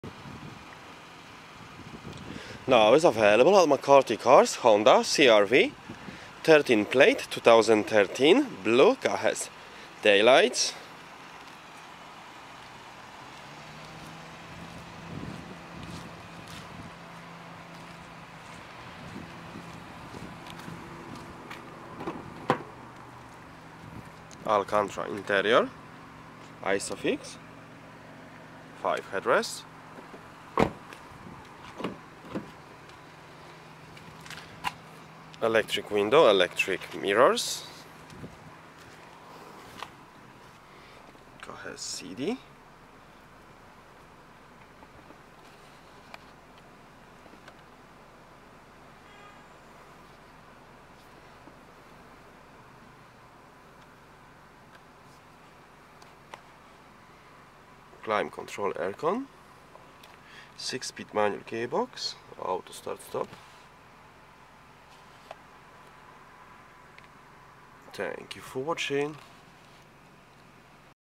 BK13ETL Honda CRV 2.0 i-VTEC S SUV 5dr Petrol Manual 2WD Air Conditioning, Cruise Control, Stereo
BK13ETL
Honda  CRV 2.0 i-VTEC S SUV 5dr Petrol Manual 2WD Air Conditioning, Cruise Control, Stereo, CD Input, USB/AUX Input, NO ULEZ CHARGE.